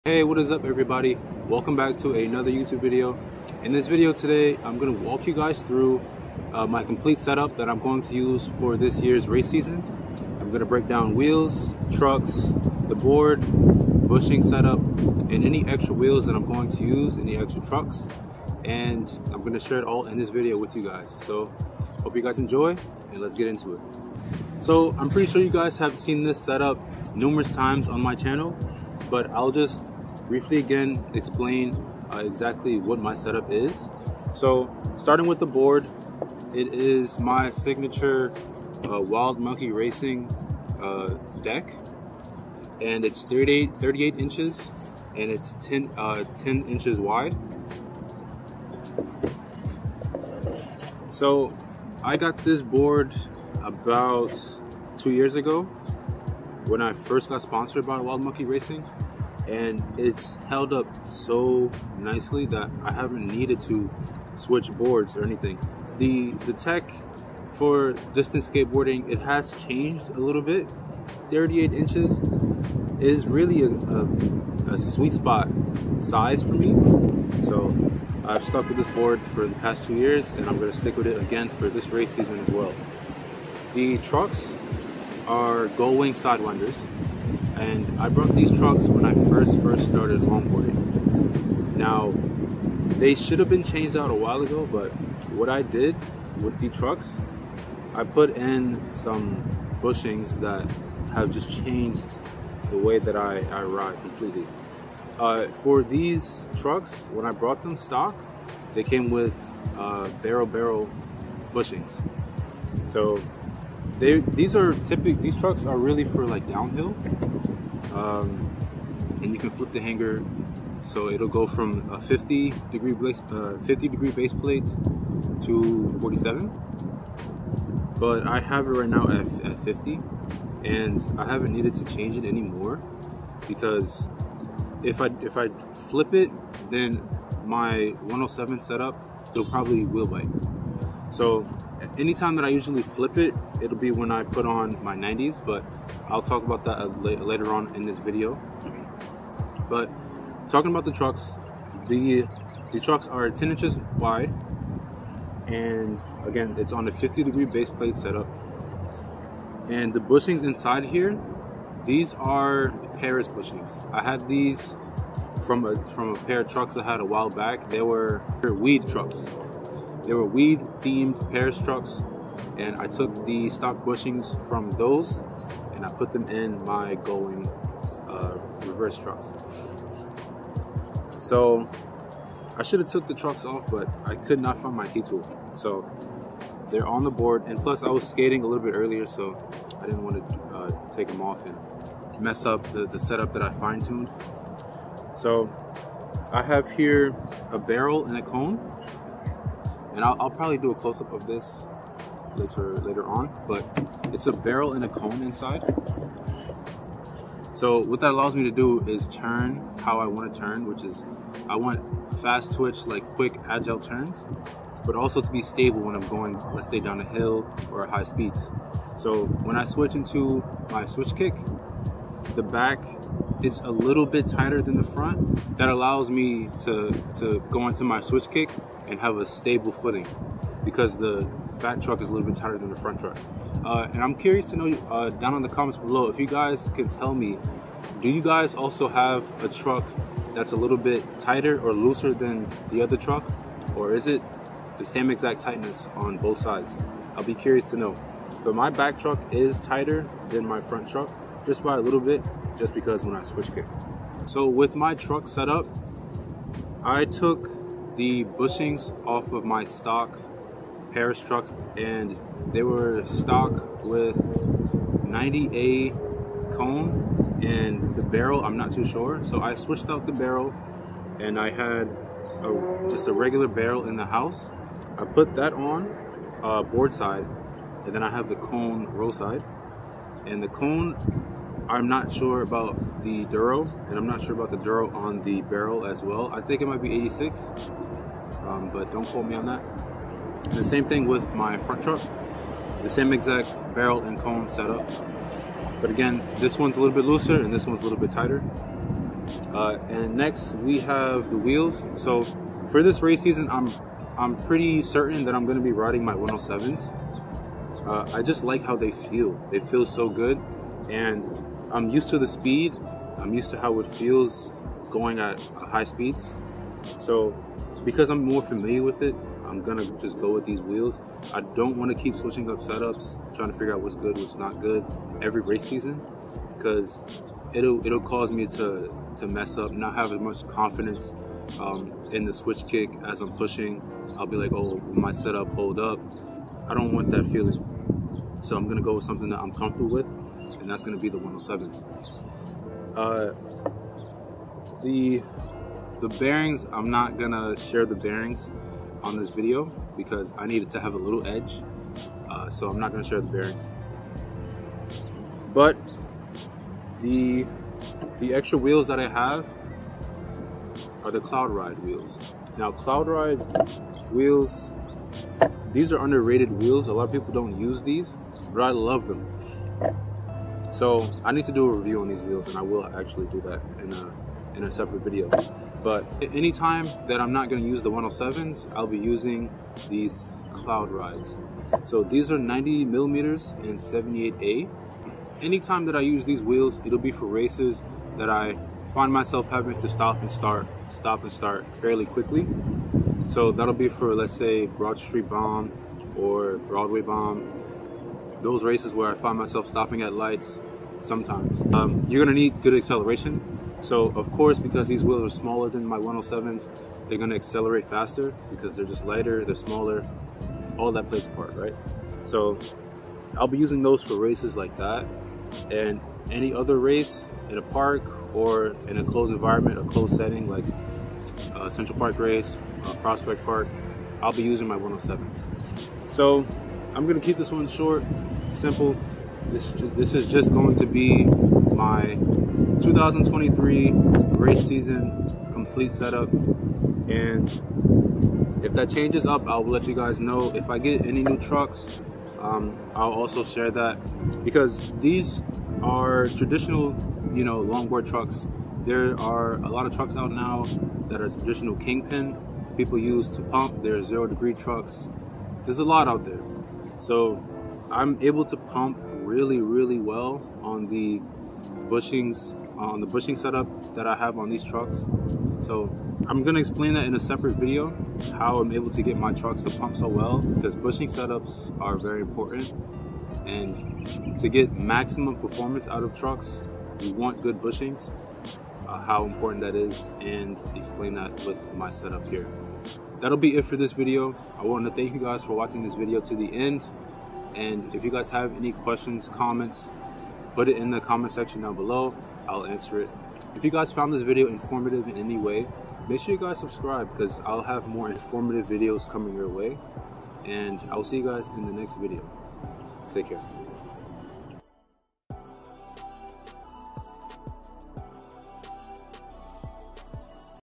My 2023 Race Season Complete Setup | Distance Skating
In this video, I go over the setup that I will be using for this year's race season. I talk about everything I will use and how that helps me to be so fast.

SOCIAL MEDIA//